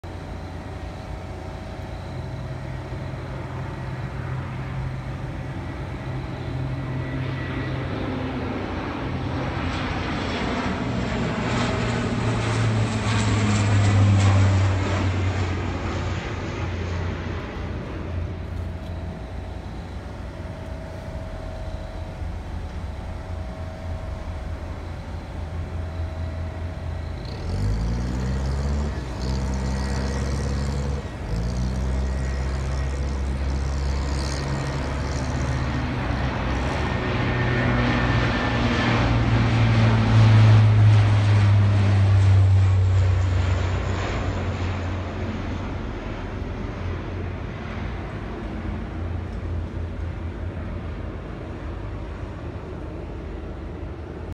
Two Royal Air Force C130J ILS Approach into Leeds Bradford Airport EGNM
Two C130J Carrington out formation ILS approach for training into Leeds Bradford Airport.